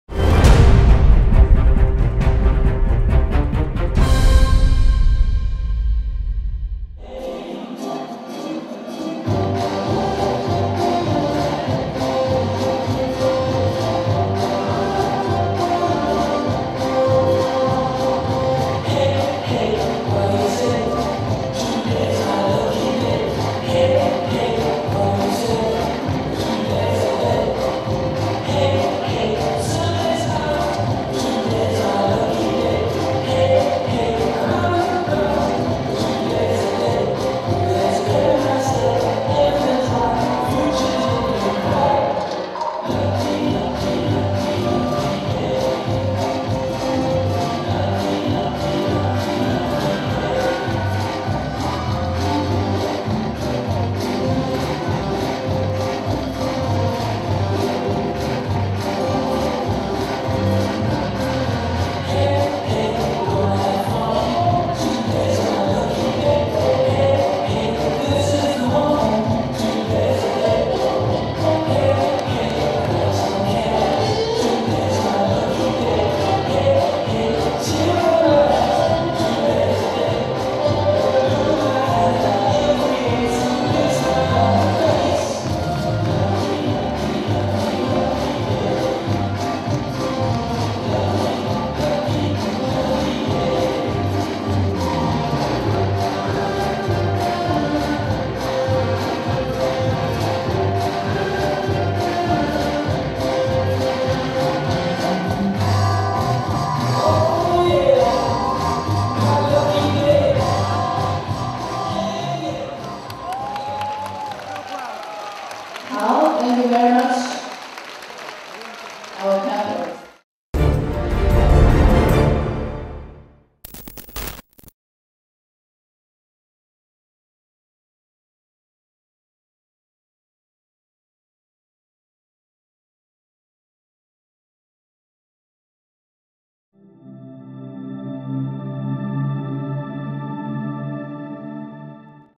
Barden - de Fonseka, AUS | 2015 World 10D R2 Q | DanceSport Total
Barden - de Fonseka, AUS | 2015 World 10D R2 Q

Brodie Barden - Lana Skrgic- de Fonseka, AUS, dance the Quickstep in round 2 of the 2015 World Ten Dance in Chengdu, CHN, on 5 September.